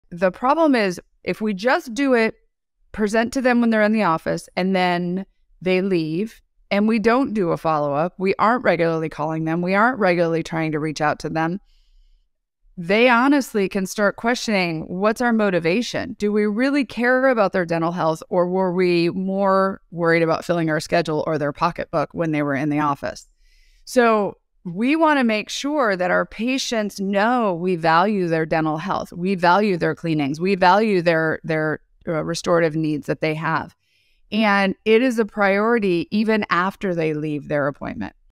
Dear Laura Show - Ep 7 - H3- Overdue recare and outstanding calls | Explaining balance to a patient
Are you struggling with managing overdue repairs and outstanding treatment plan calls at your dental practice?

Tune in to the latest podcast episode of the Dear Laura show, where Laura shares her expert insights on how to handle these issues with ease.

In this episode, Laura emphasizes the importance of regular patient outreach to keep them engaged and committed to their dental health. By leveraging metrics software, emails, and text messages, you can easily stay on top of appointments and prevent patients from falling out of the schedule.

Don't let your patients slip through the cracks! Join us as we dive into the best practices for keeping your dental practice running smoothly!